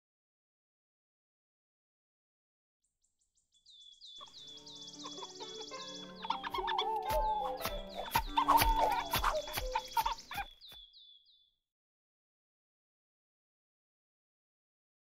Trickfilm-Waldkulisse aus Umzugskarton
Frohe Ostern 2011: Eine kurze Test-Kamerafahrt entlang einer Trickfilm-"Wald"-Kulisse, gebaut aus einem (halben) Umzugskarton...